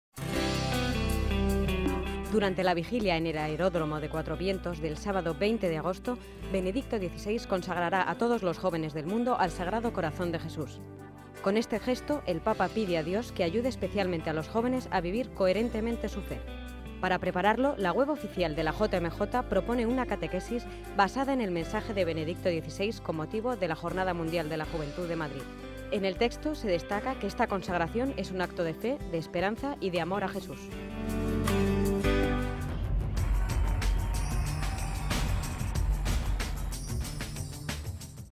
El Papa consagrará los jóvenes al Sagrado Corazón de Jesús durante la JMJ Madrid 2011
1 de julio, 2011. (Romereports.com) Durante la Vigilia en el aeródromo de Cuatro Vientos del sábado 20 de agosto, Benedicto XVI consagrará a todos los jóvenes del mundo al Sagrado Corazón de Jesús.

Con este gesto, el Papa pide a Dios que ayude especialmente a los jóvenes a vivir coherentemente su fe.

Para prepararlo la web oficial de la JMJ propone una catequesis basada en el mensaje de Benedicto XVI con motivo de la JMJ de Madrid. En el texto se destaca que esta consagración es un acto de fe, de esperanza y de amor a Jesús.